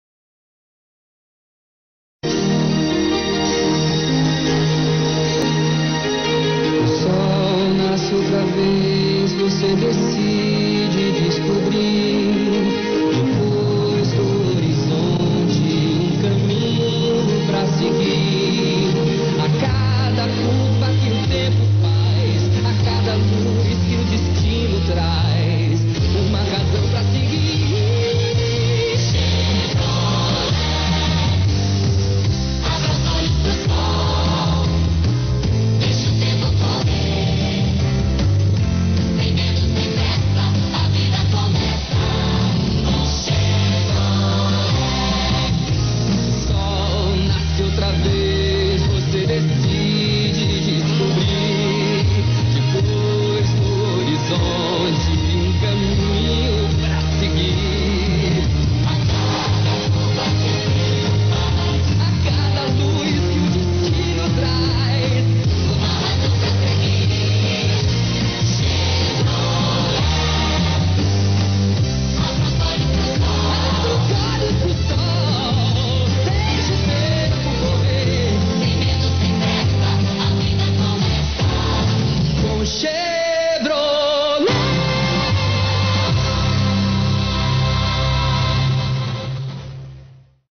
Videoclip Chevrolet Opala Diplomata 1988-1990
Se muestran clips de videos del Chevrolet Diplomata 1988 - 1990, acompañada de una canción que apareció en una publicidad de los modelos de GM do Brasil en 1990. Los Videos se tomaron del video del Diplomata Collectors